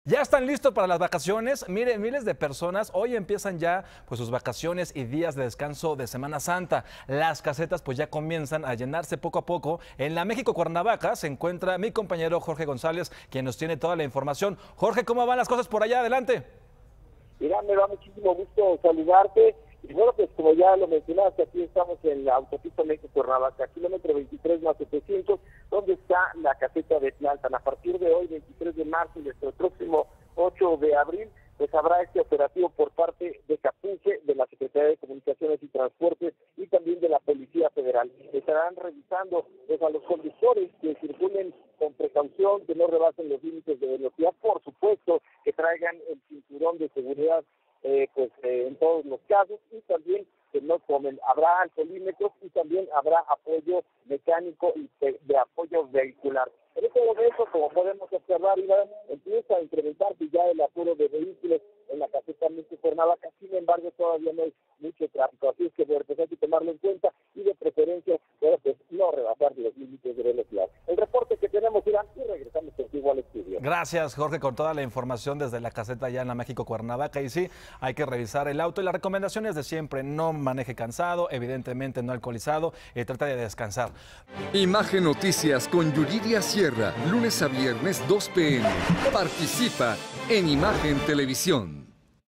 ¡Ya casi son vacaciones! las casetas se empiezan a llenar | Noticias con Yuriria Sierra
En la carretera México- Cuernavaca se empieza a llenar.